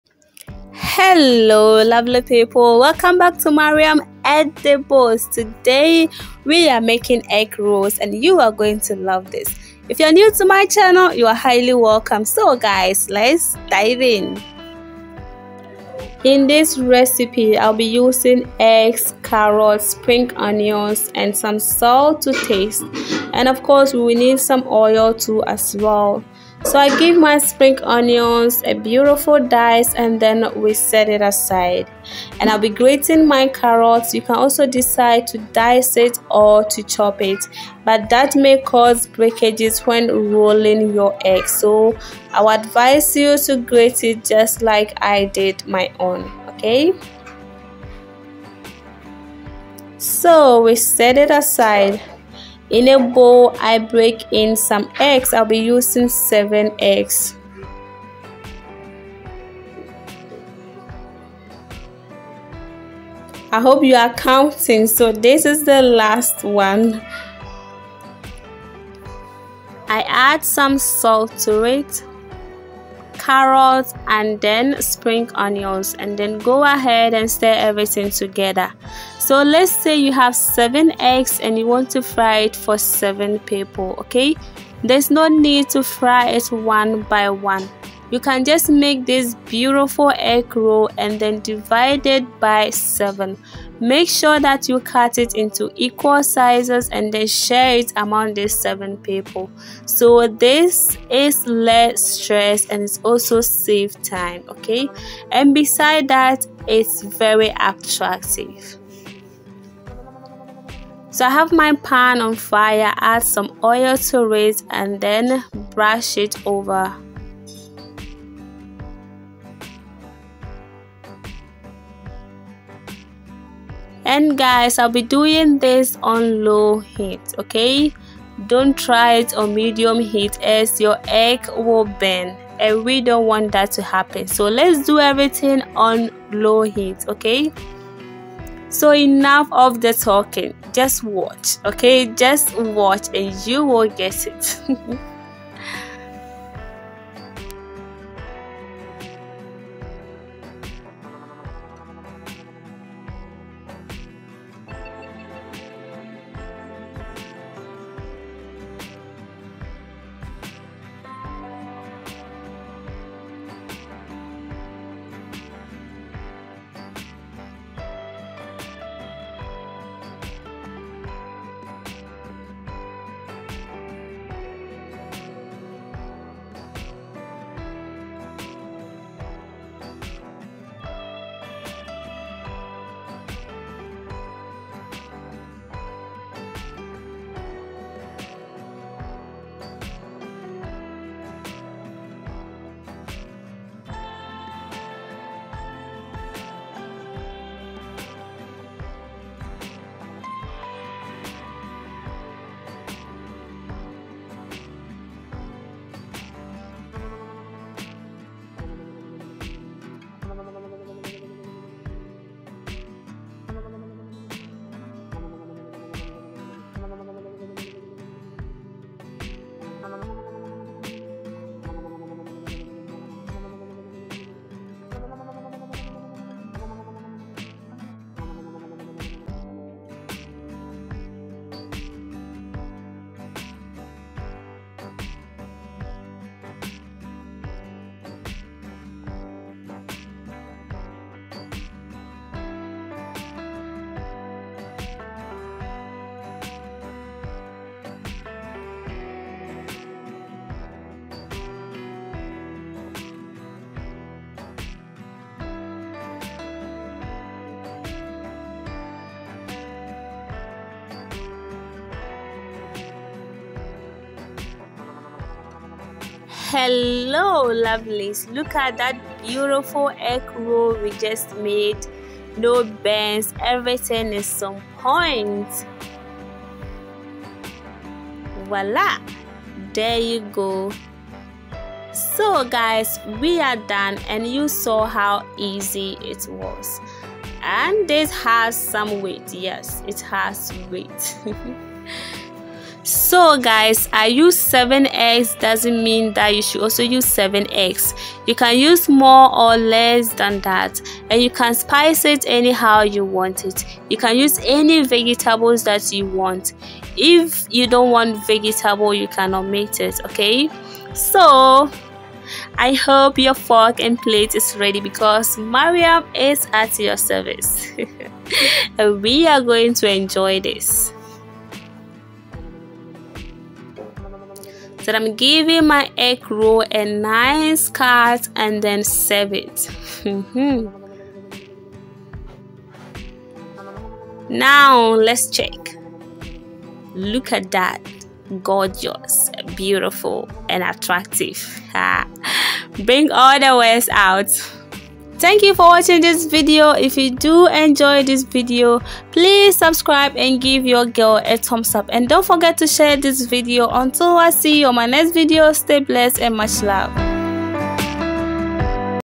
how to make egg rolls at home
this is a step-by-step tutorial on how to make egg roll at home. it's very simple and easy to make

INGREDIENTS
eggs
carrot
spring onions
salt
oil( you can also use butter)